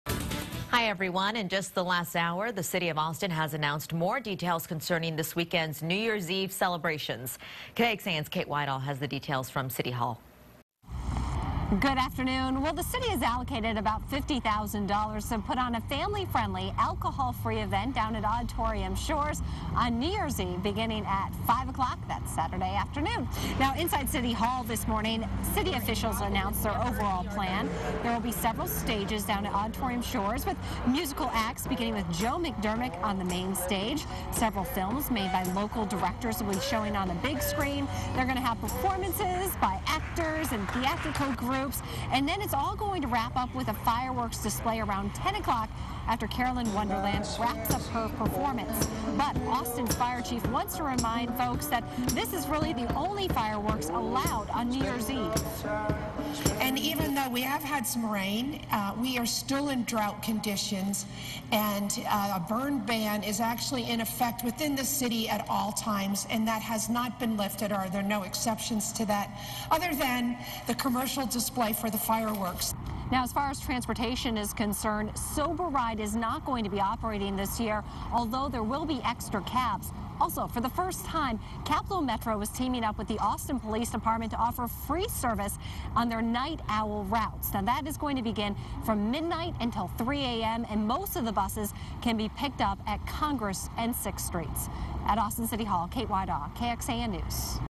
Family-friendly New Year party planned
The city of Austin said Wednesday it is allocating $50,000 to help throw a family-friendly New Year's Eve Party.